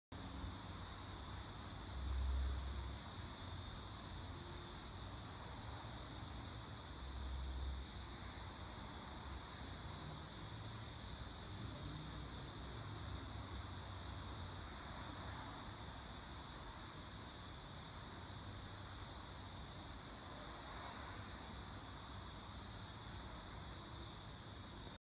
Moon and Venus? 3/12/2016 Melb Australia
So is it Venus?... is that the Moon?